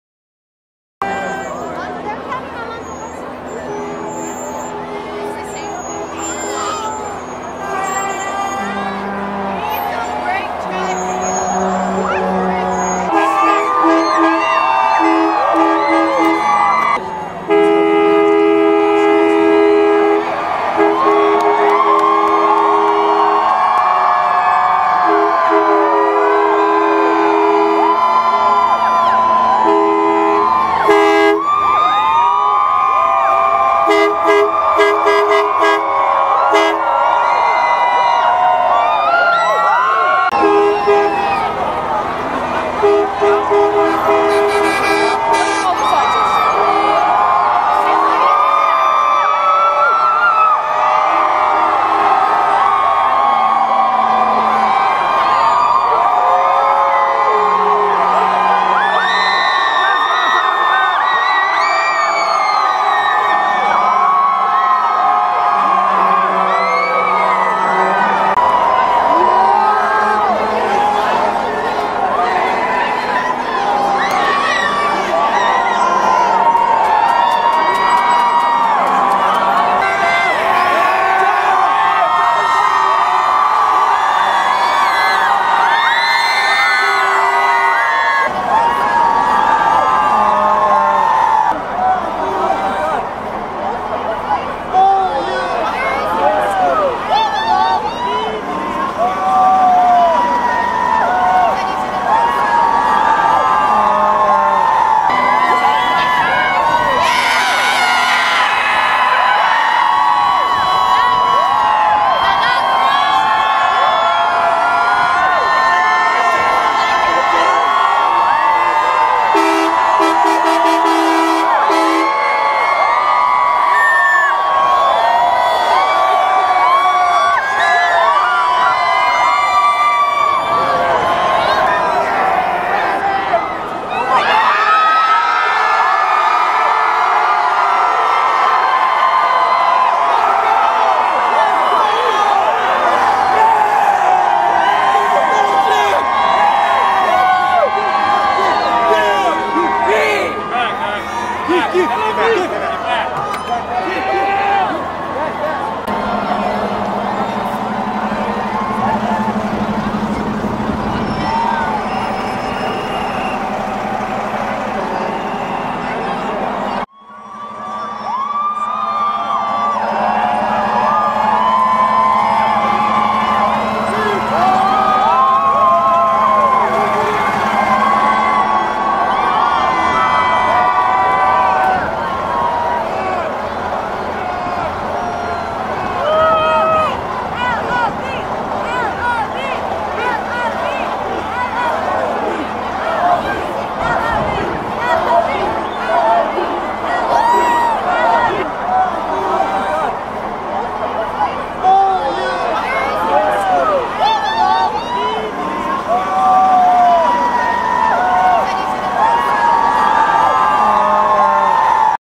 Seahawks Parade Beginning Part 1 James Street Seattle 12th Man Mobile
Seahawks Parade Part 1, Beginning Police Escort & Buss's Near James Street Seattle From 12th Man Mobile.
Part #2 loaded up soon... with 6 blocks filming of Marshawn Lynch and crowd.